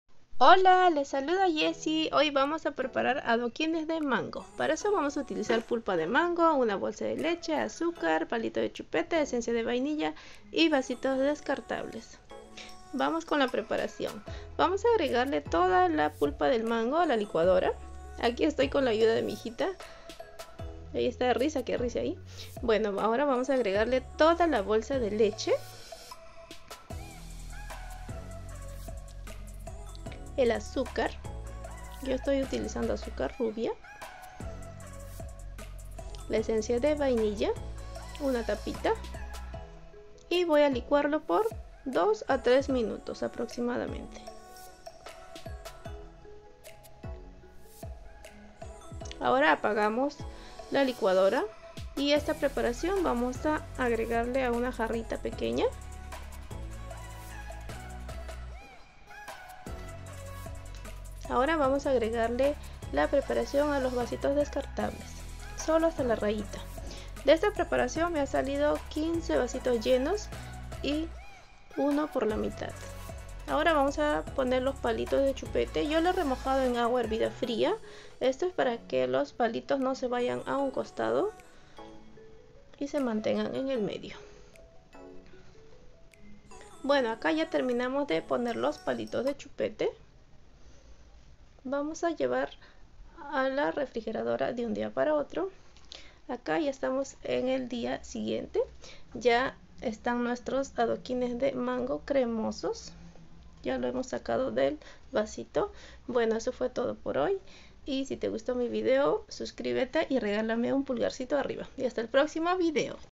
adoquines de mango cremosos 🥭🥭
ingredientes : para 15 vasitos
pulpa de mango    750 gr
una bolsa de leche 900 ML
azúcar rubia            200 gr
palitos de madera
esencia vainilla 1 tpt
vasitos descartables de 5oz 15